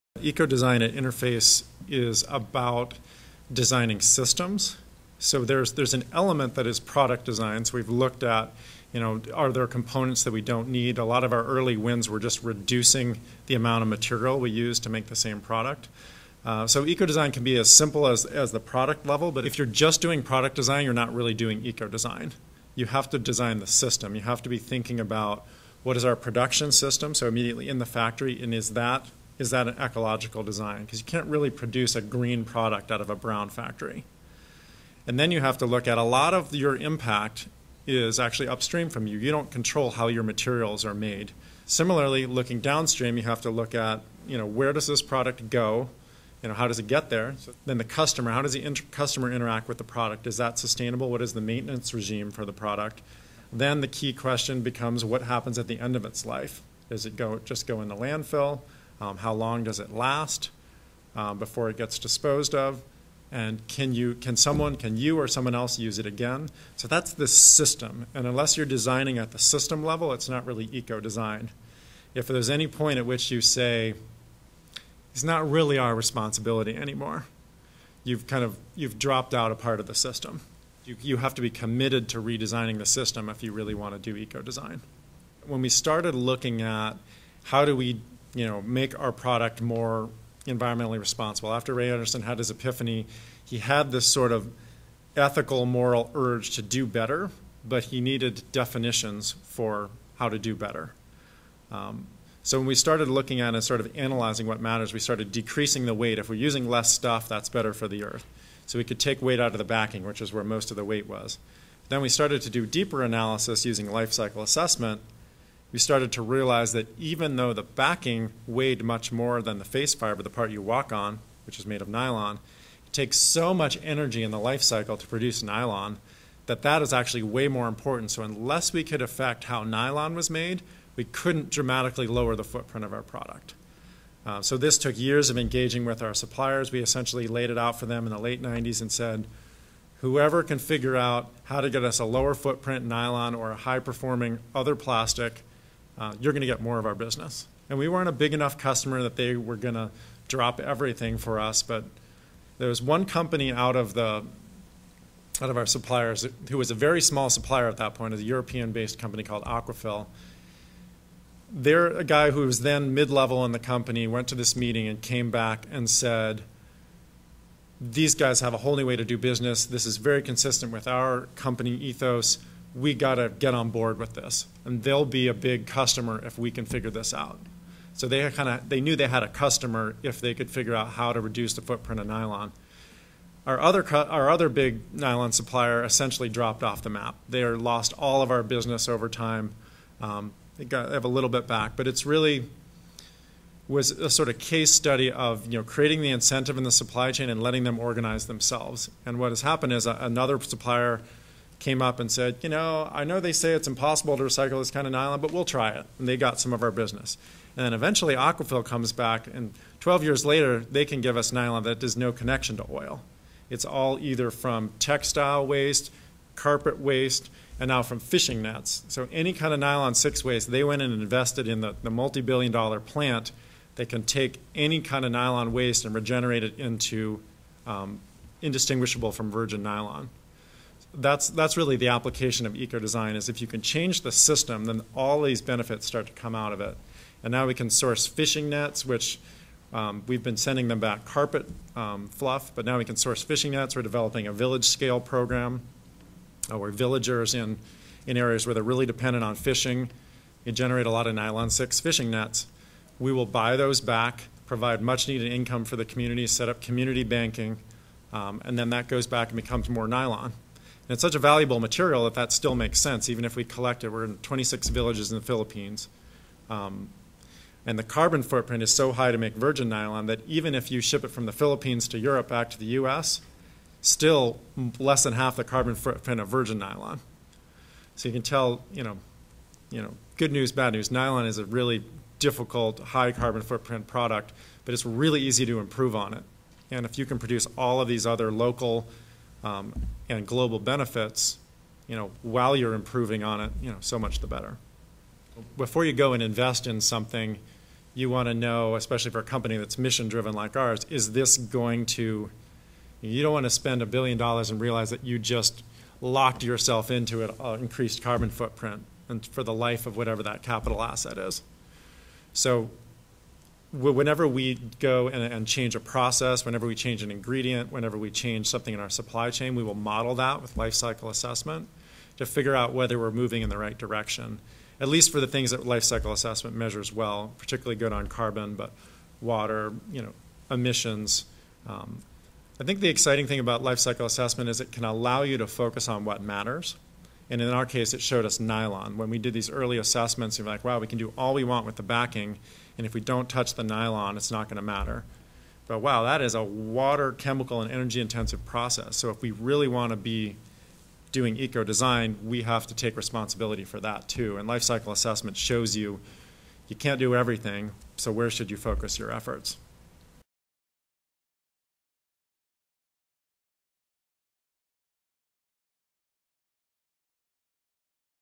AGRION Interview with Mikhail Davis, Director of Restorative Enterprise at Interface
On March 18,2014 AGRION sat down with Mikhail Davis from Interface to discuss the definition, application, and impacts of eco-design on both the company he works for and the products it makes.